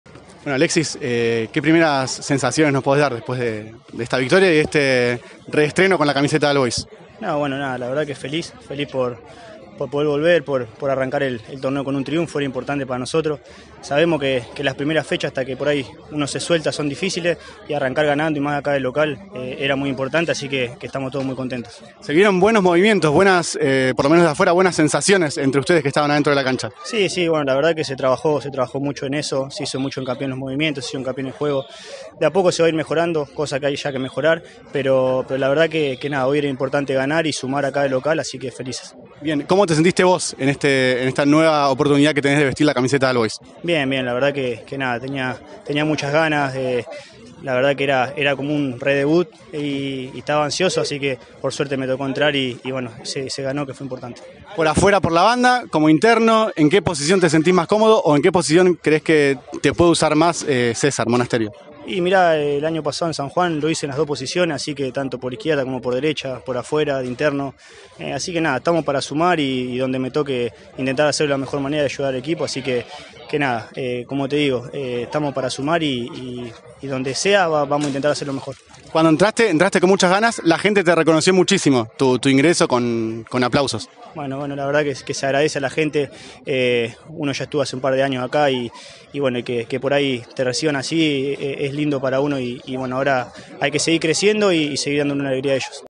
ENTREVISTA A ALEXIS VEGA - ALL BOYS 1 VS TRISTAN SUAREZ 0 - PN 2024 - FECHA #1
Entrevista a Alexis Vega tras la victoria de AllBoys por 1 a 0 frente a TrsitanSuarez en el debut del conjunto de Monasterio en esta Primera Nacional.